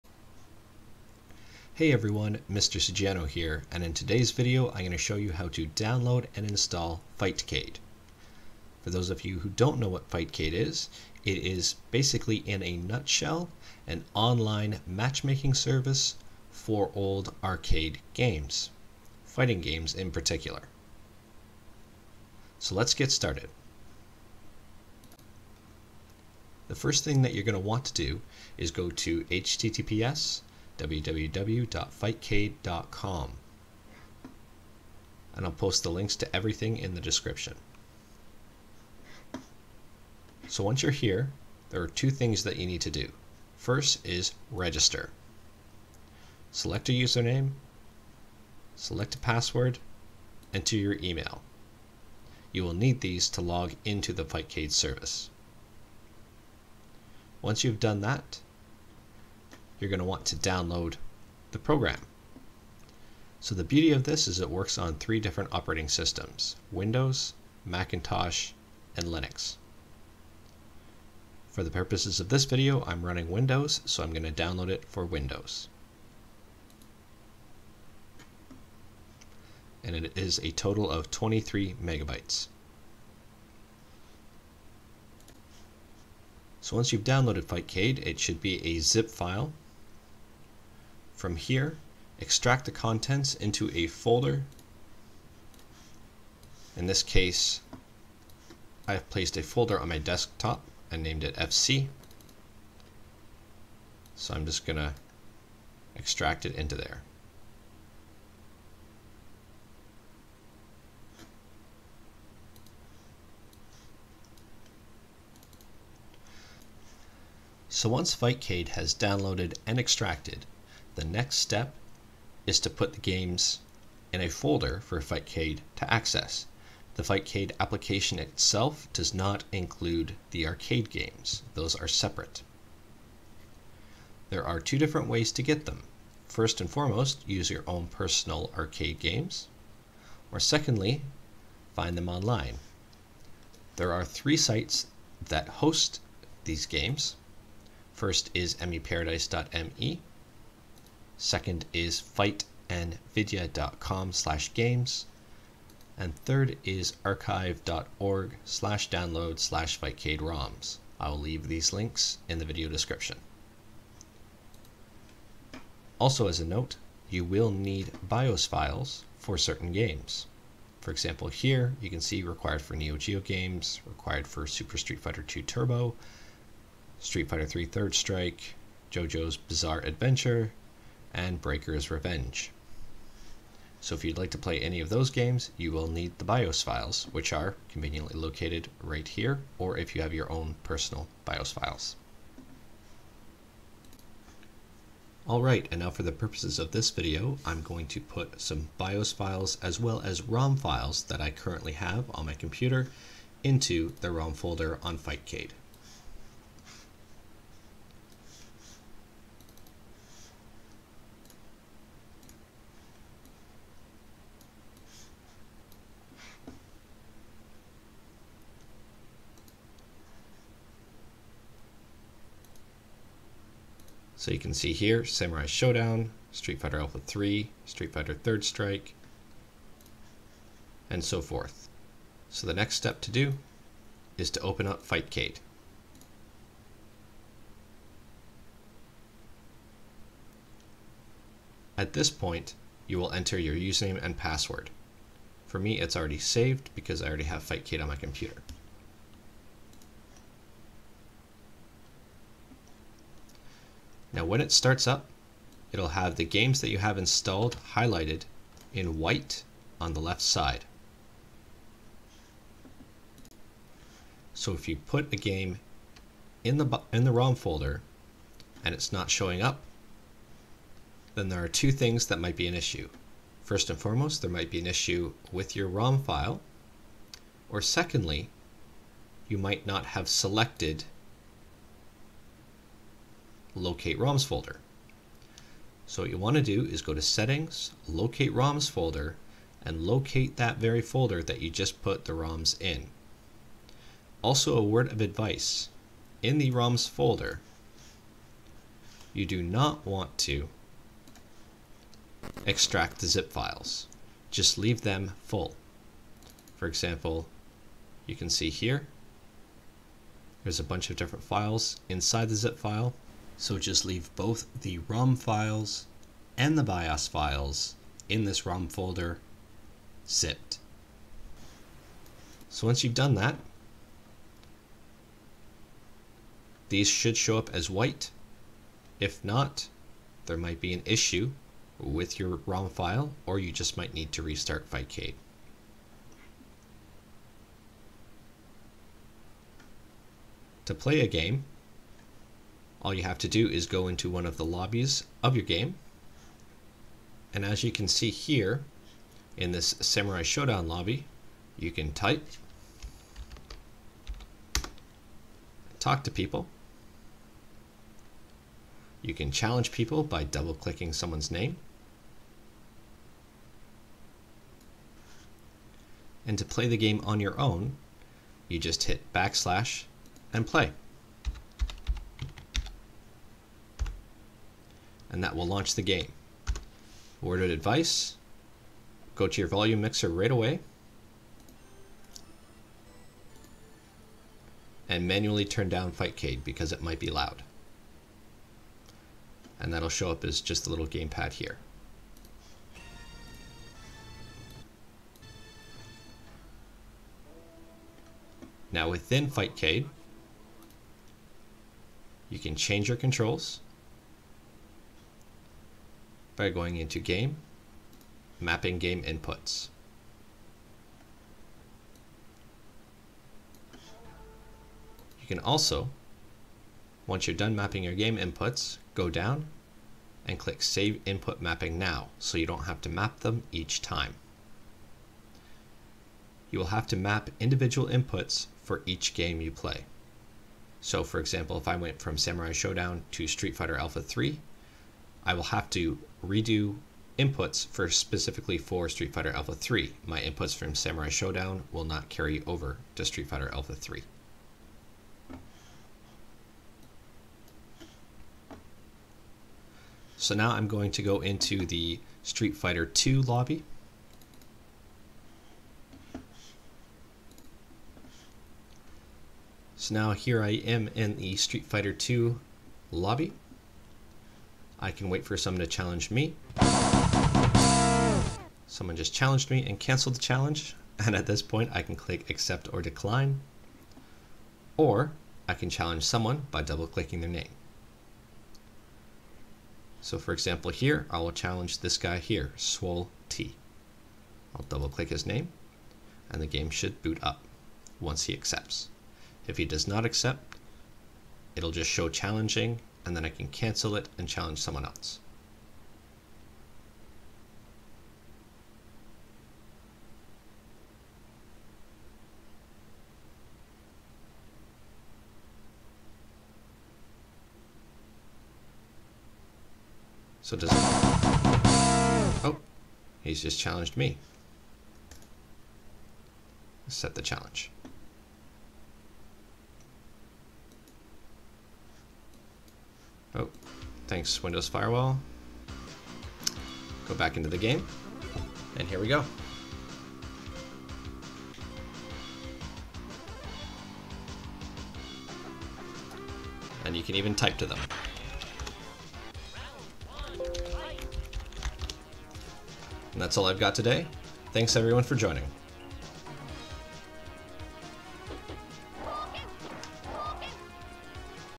Fightcade Walkthrough - How-to / Installing / General tips
In this video I provide a Fightcade walkthrough, with How-to / Installing / General tips